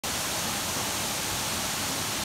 霧島観光名所の丸尾滝①（2020年12月）
鹿児島県霧島温泉街の近くにある丸尾滝です。
近くの栄之尾温泉、硫黄谷温泉の温泉水が入り交じった
乳白色した珍しい滝です。